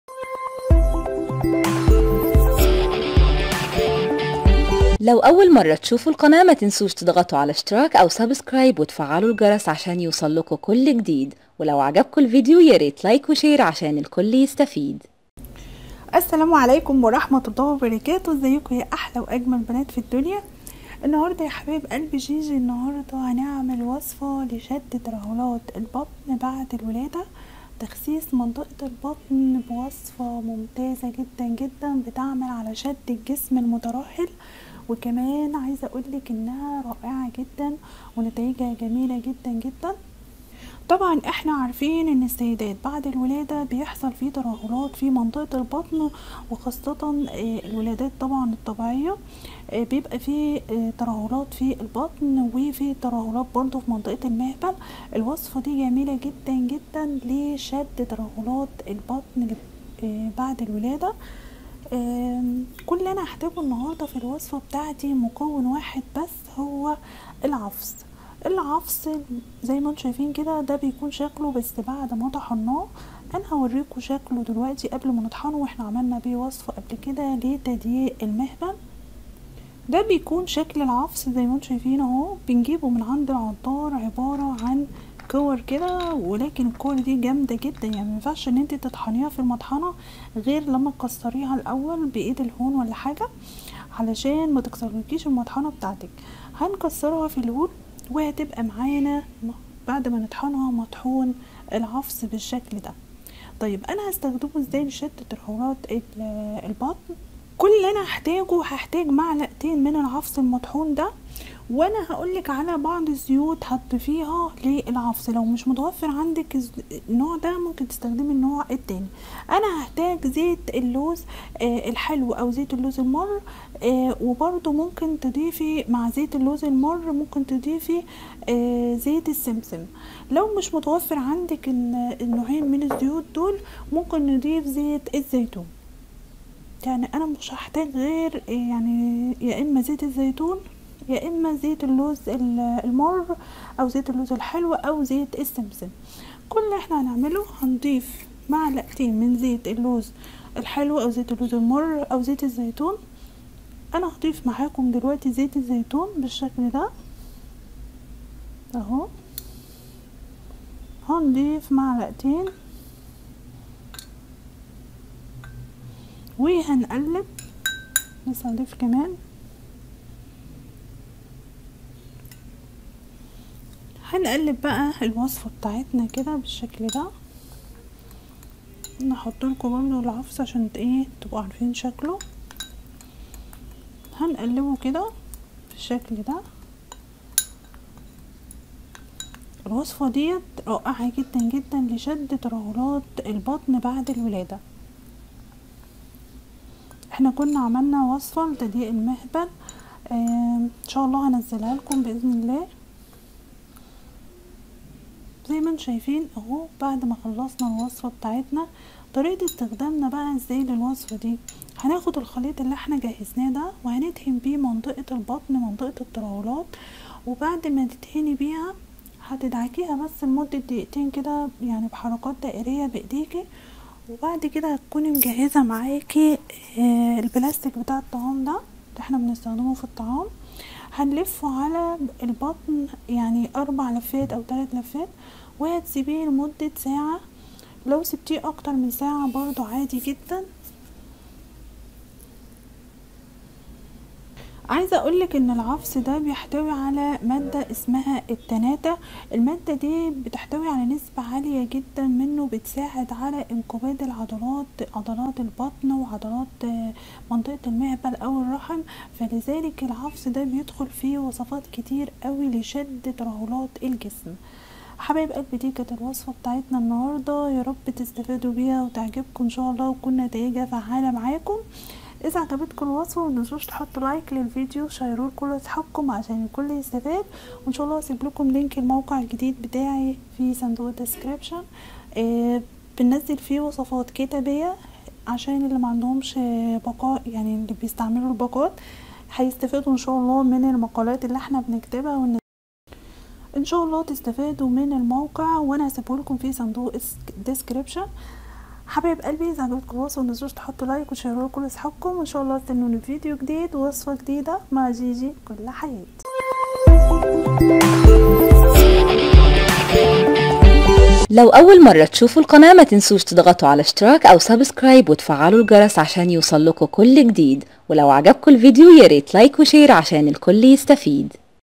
ضعيها علي بطنك نصف ساعه لاحظي كيف تذوب الدهون وتتخلصي من الكرش نهائيا دهان تخسيس الكرش
فيديو النهاردة لتخسيس منطقة البطن وكمان شد الترهلات بعد الولادة بمكون واحد فقط شاهدى الفيديو ومتنسوش تشتركو بالقناة وتفعيل الجرس